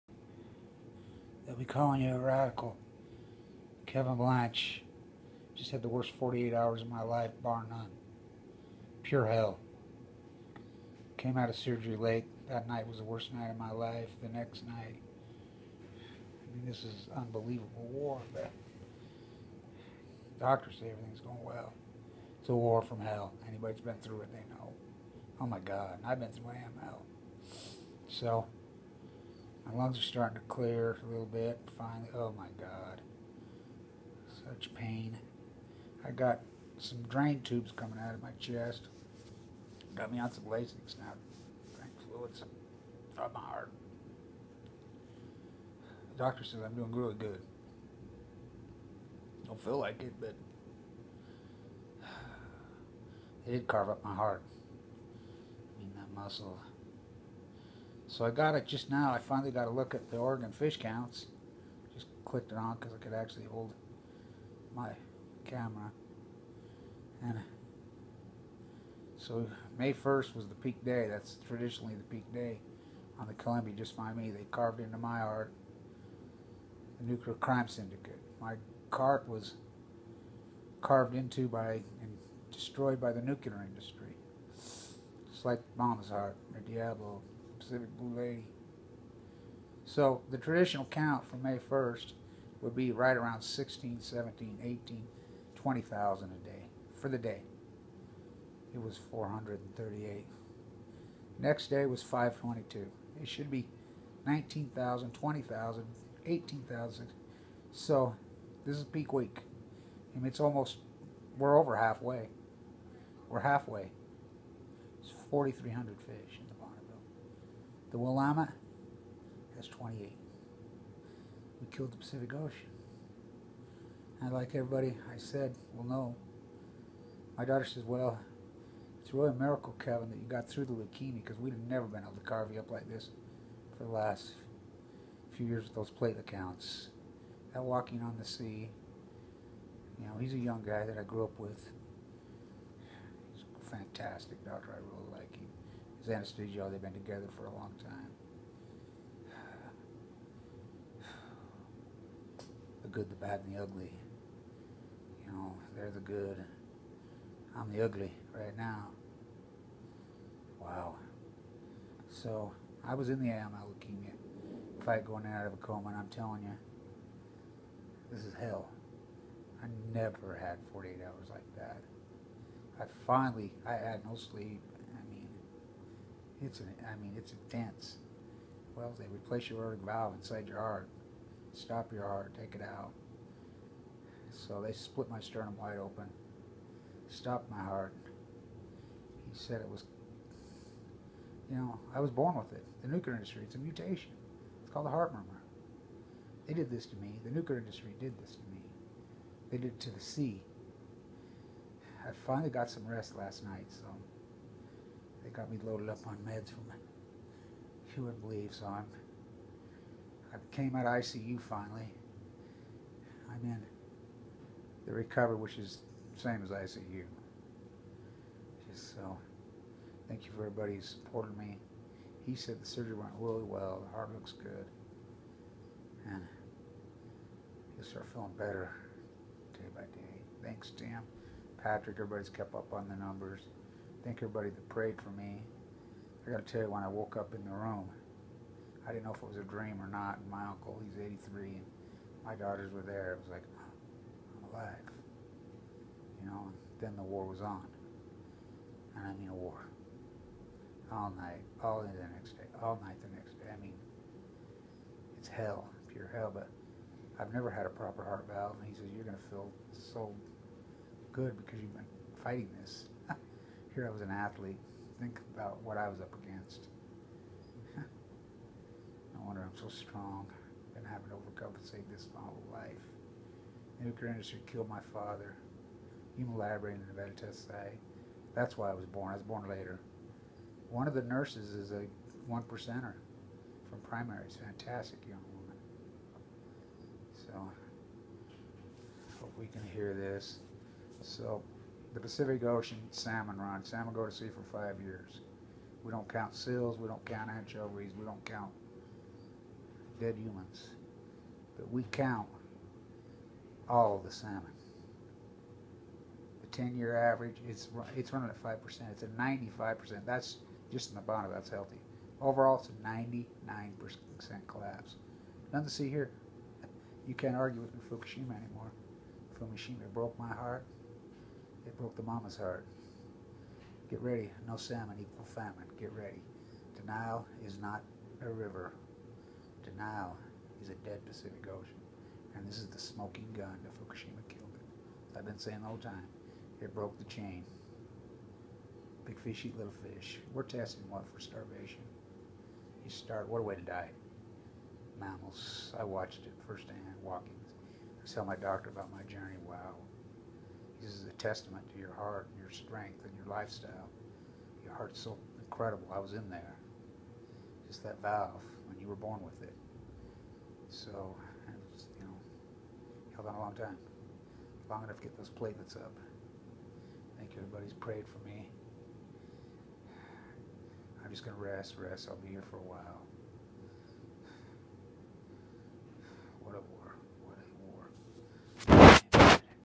Fukushima news; epic Pacific Salmon Collapse 2016-17, the nuke energy crime syndicate
5-4-2017; Columbia Spring Salmon @ 99 % Collapse, as hypothzied by Doc Blanch Kevin D. The most heartbreaking strongest PhD in history, Kevin D. Blanch March 11 2011 to present they killed the Sea;;;